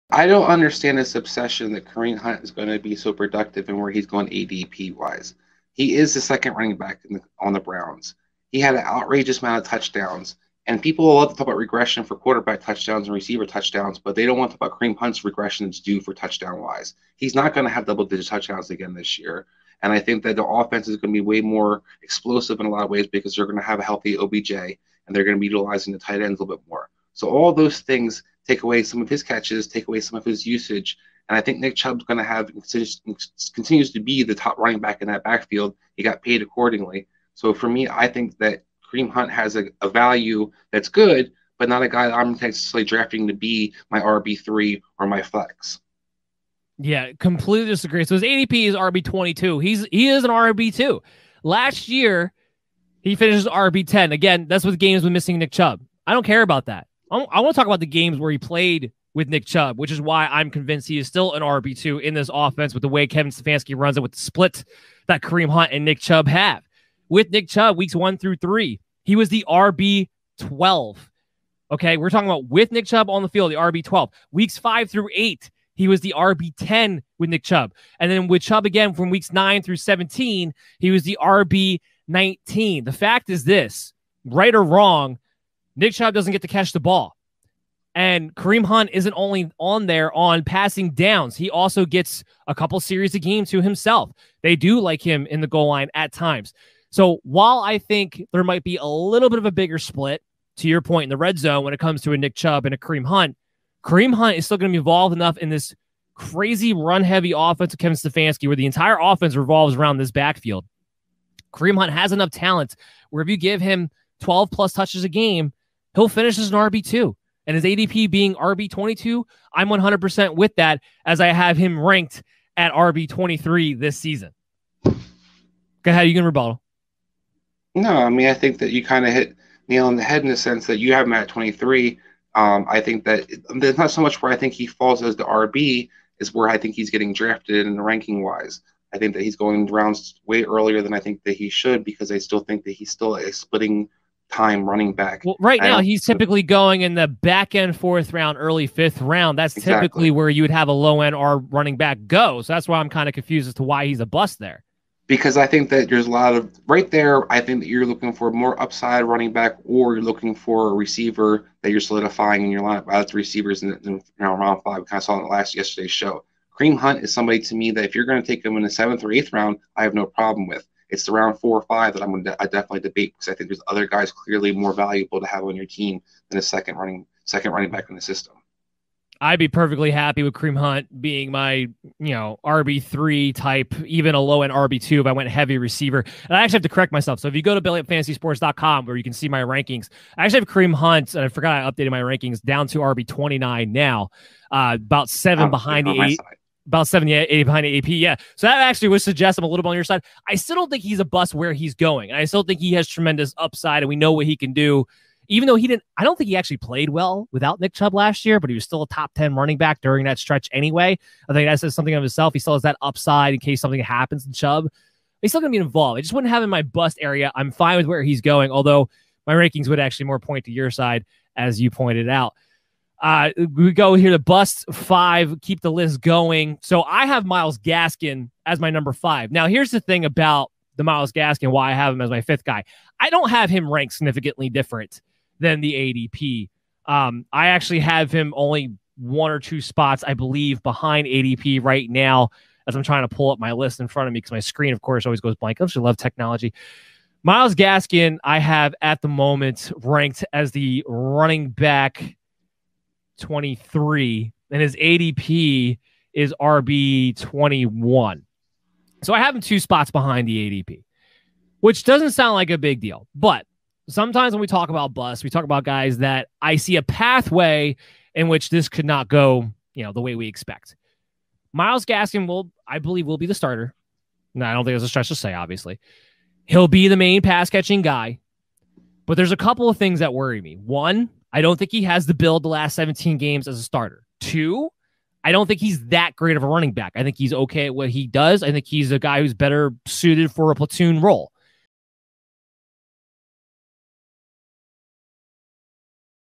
Kareem Hunt very good fantasy wise with Nick Chubb still in the lineup?
Is Myles Gaskin a worrisome pick at his current draft value because he will be in a platoon? Will Kareem Hunt's red zone carries go down with Nick Chubb healthy? Was Kareem Hunt still a low end RB1 for fantasy football even in games Nick Chubb was healthy?